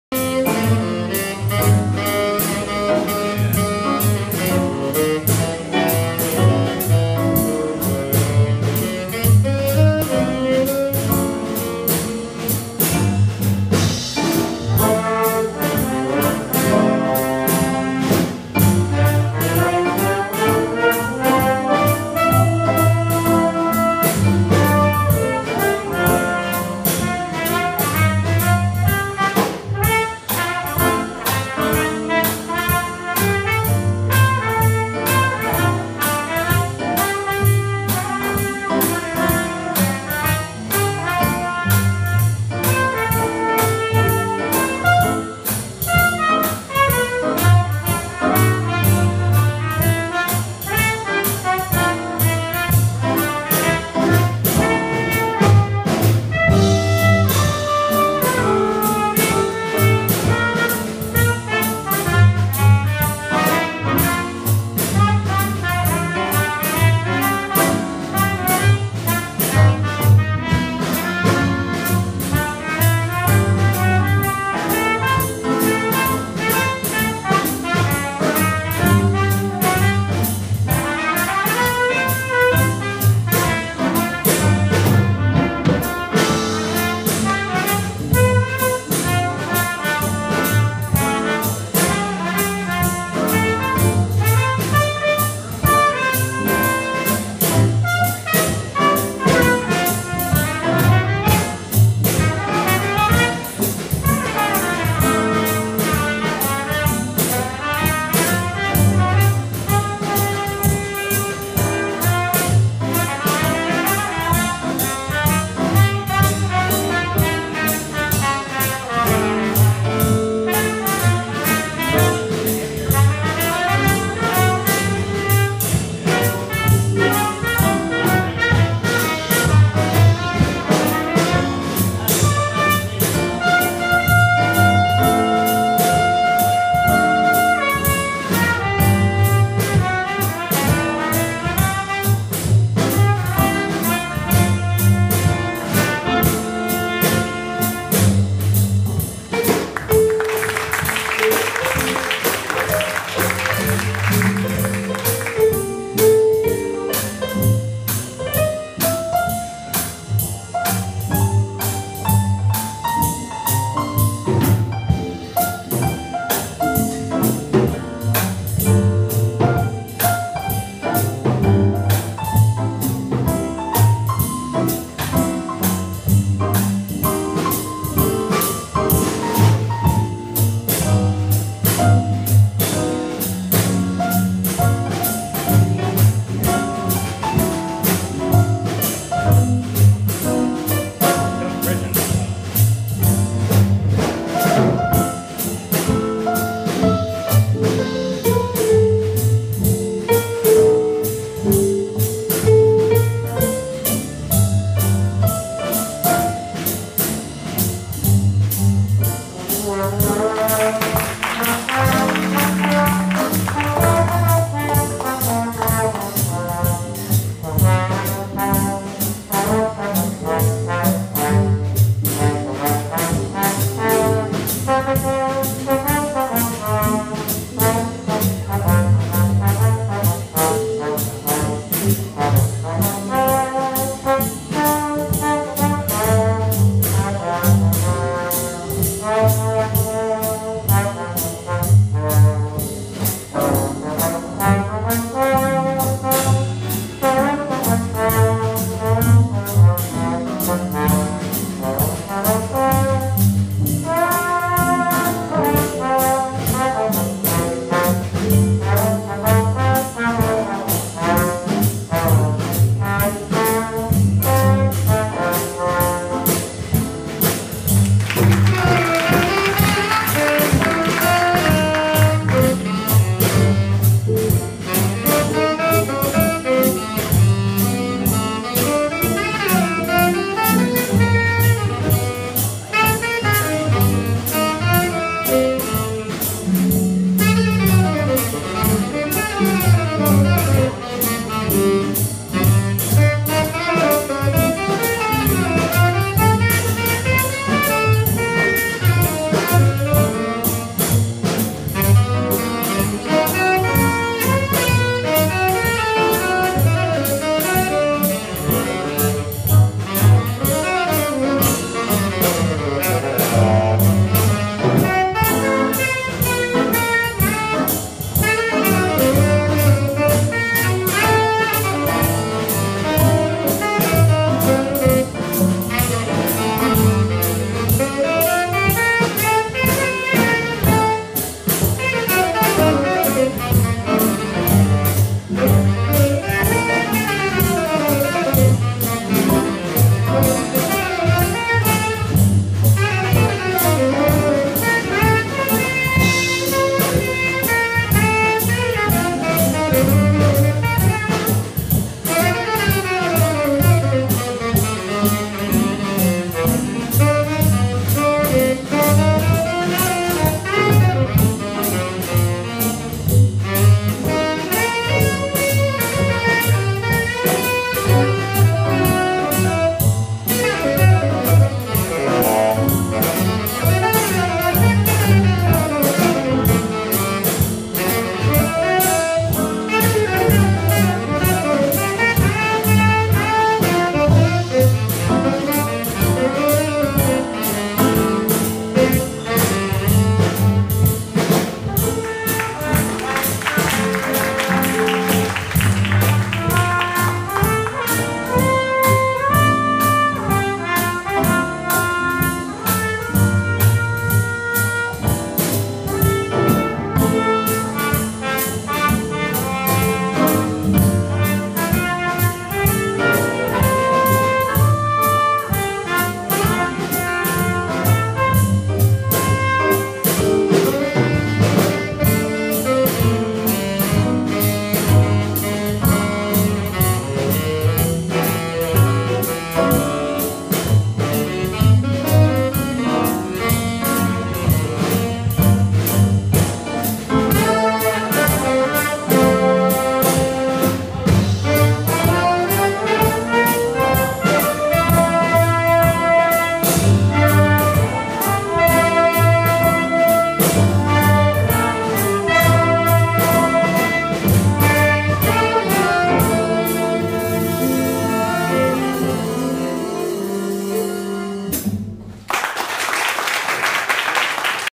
UC CCM Jazz Prep Combo at Roh's Street Cafe 4/3/14
Great performance tonight! Mitch says it was his best all year. Way to go!